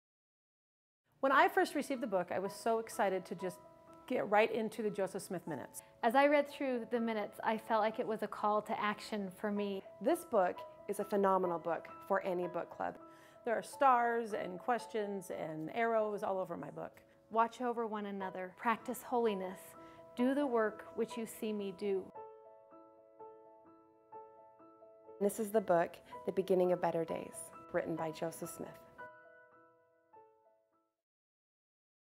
"The Beginning of Better Days" by Joseph Smith, Sheri Dew, Virginia Pearce
The Beginning of Better Days includes Joseph Smith's six timeless sermons to the women of Nauvoo, along with personal essays from Sheri Dew and Virginia H. Pearce that help us better understand the sermons and their relevance for Latter-day Saint women today.